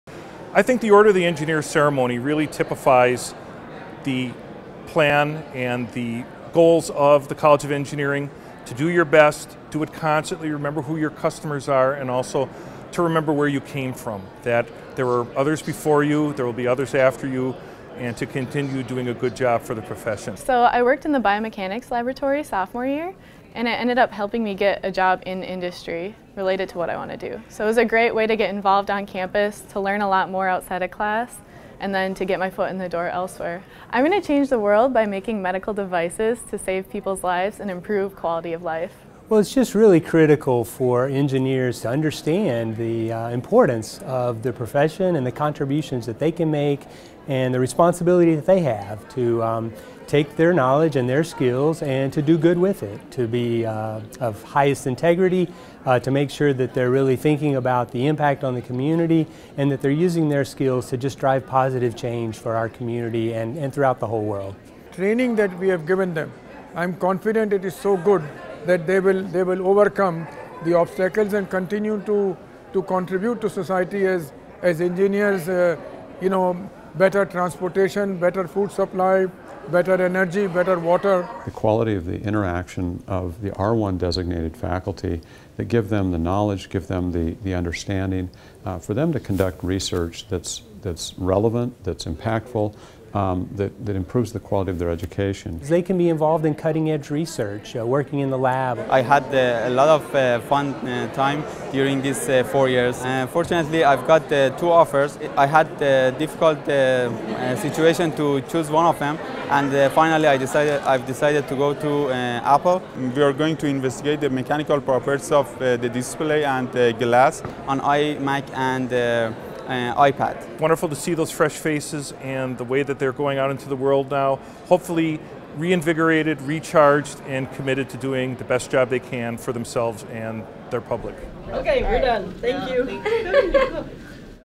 Order of the Engineer Ceremony
This special ceremony takes place each semester with each graduating class of students, fostering a spirit of pride and responsibility in the engineering profession and bridging the gap between training and experience. Congratulations, UW Milwaukee Engineers!